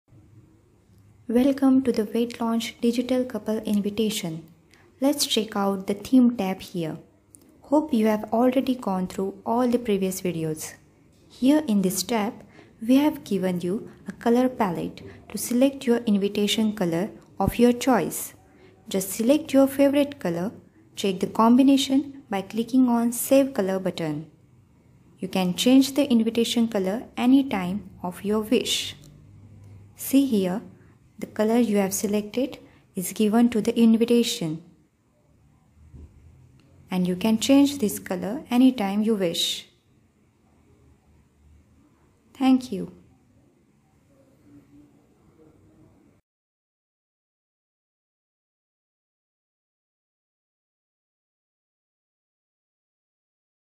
6 Theme | How to change invitation theme of your choice | The wed lounge
Create your digital couple invitation -
Check how to change invitation theme of your choice